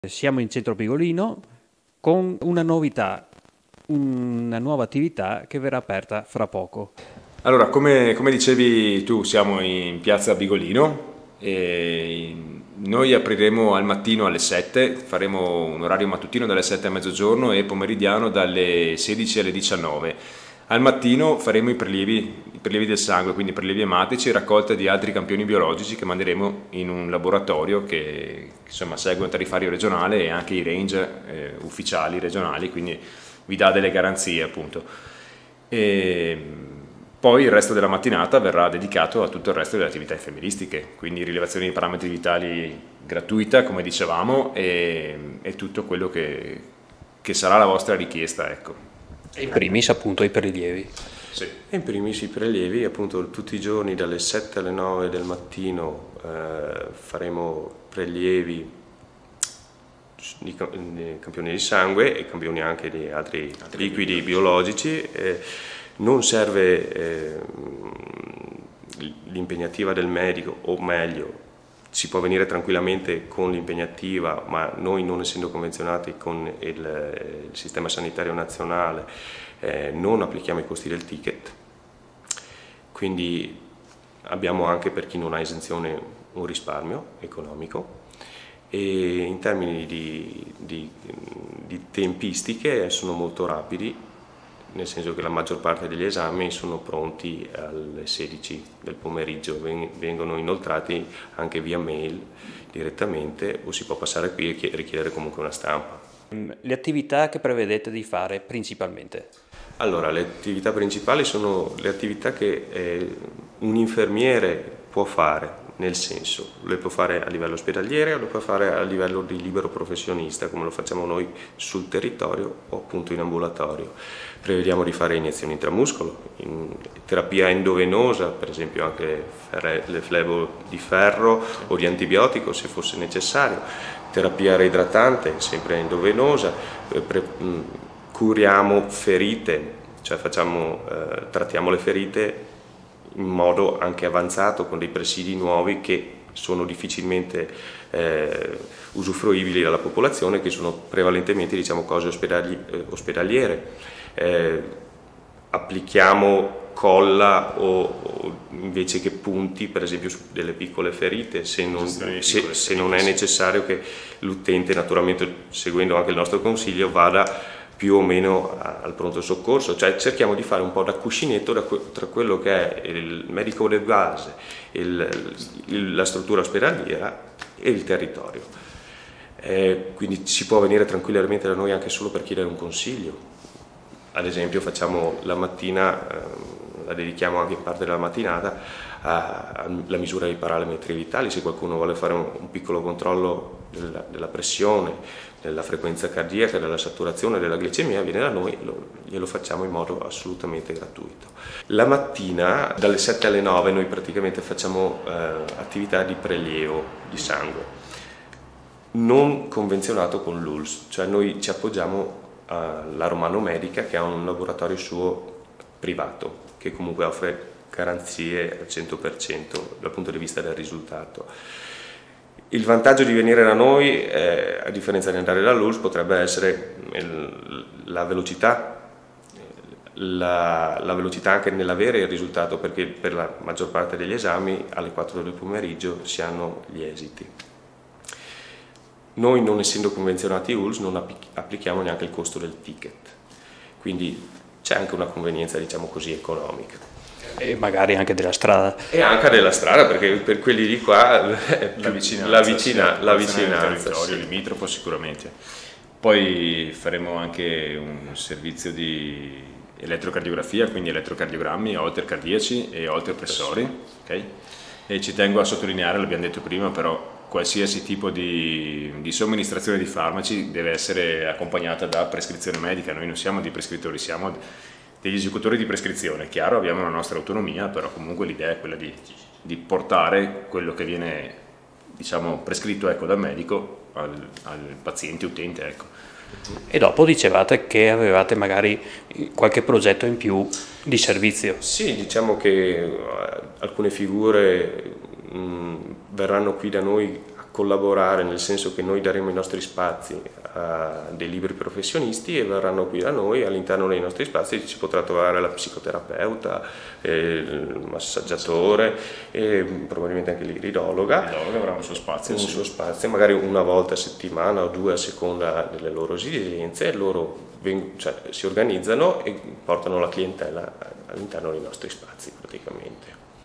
Apertura Studio Infermieristico Bigolino: Intervista
Apre a Bigolino di Valdobbiadene, TV un nuovo studio infermieristico e centro prelievi; un'iniziativa di due giovani infermieri, Luca De Rossi e Marco Rolli con lo scopo di dare dei servizi utili al completamento delle richieste del pubblico.

Nell'intervista i due soci spiegano che cosa si può trovare, i servizi offerti e quelli che verranno nel prossimo futuro.

Servizio a cura di:
Sergio Capretta